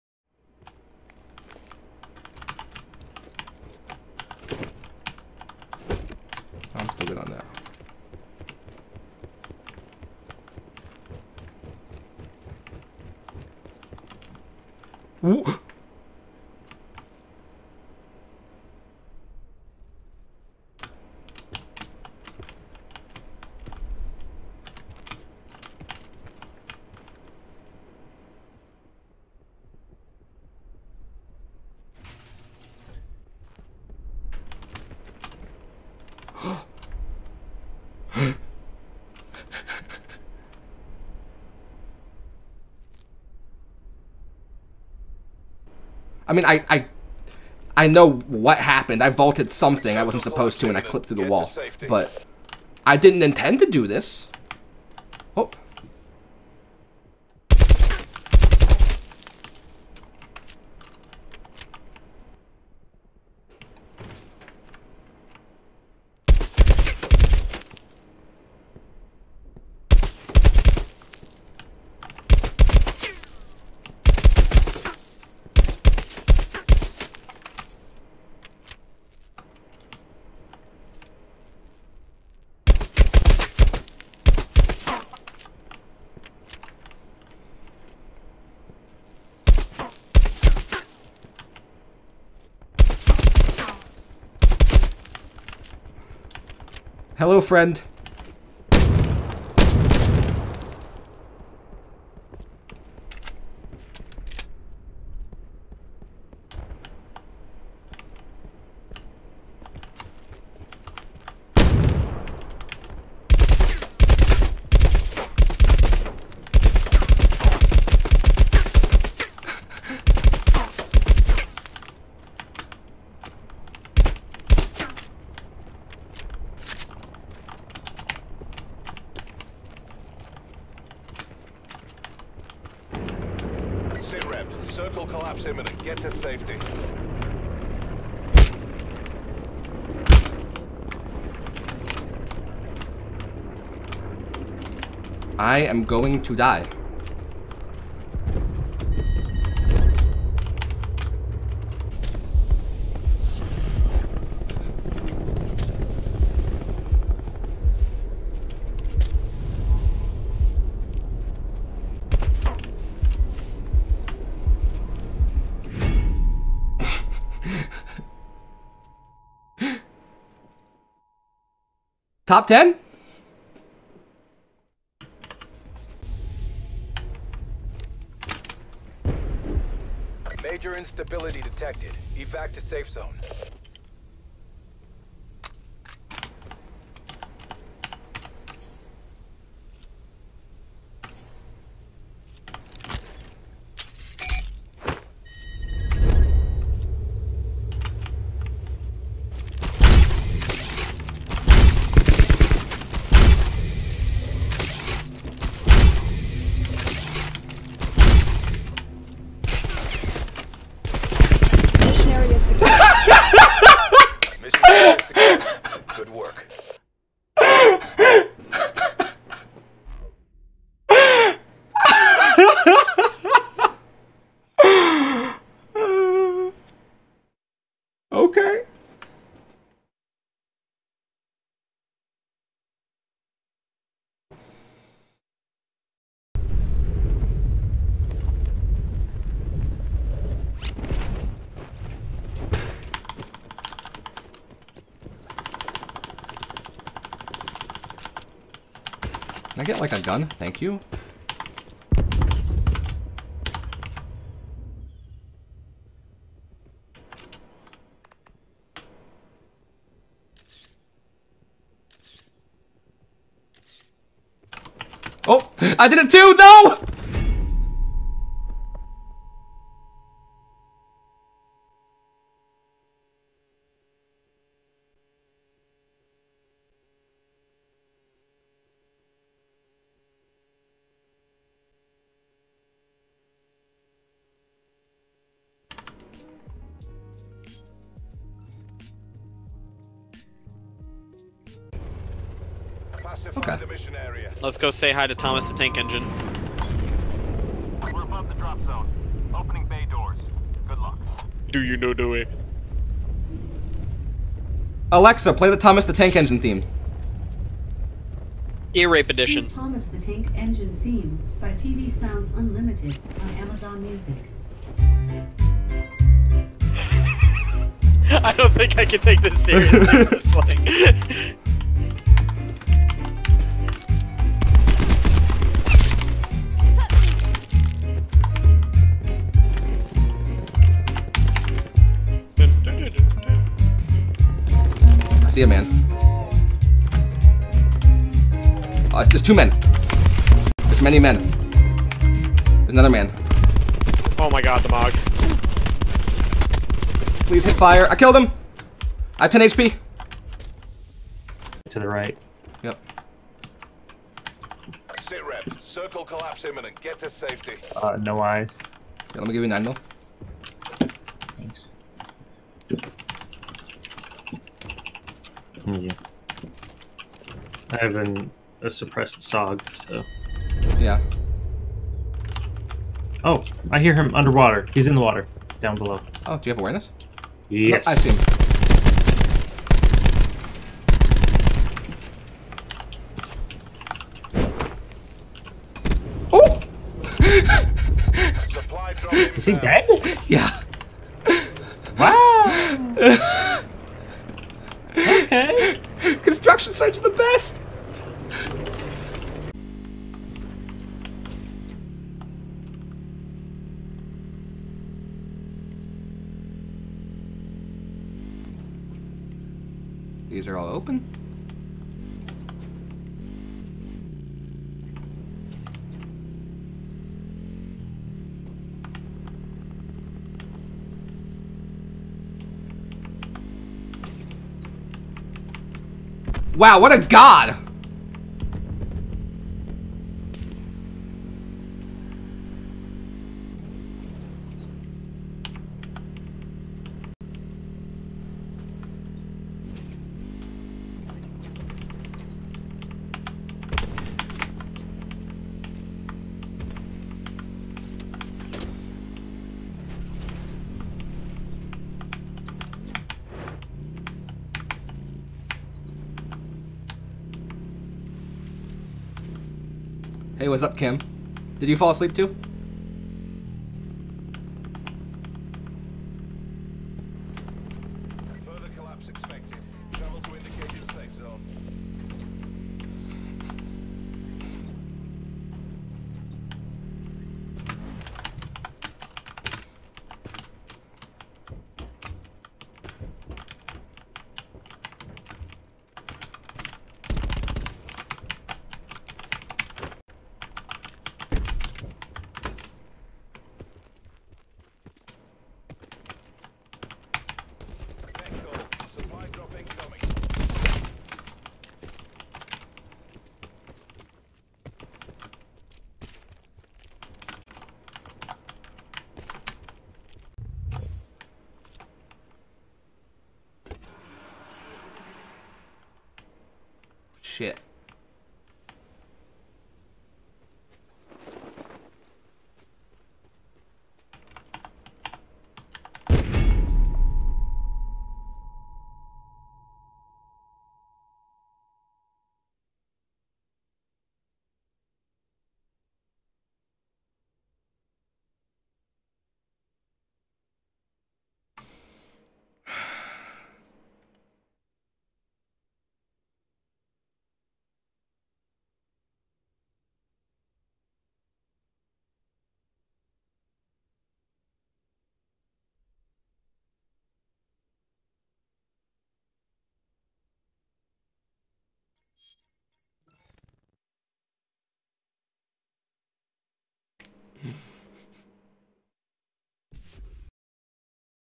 Glitches & Fails - Blackout Stream Highlights Episode 2
All clips taken from my stream, from October 18th to November 13th.  This episode is mostly weird and silly clips, but next episode is coming very soon.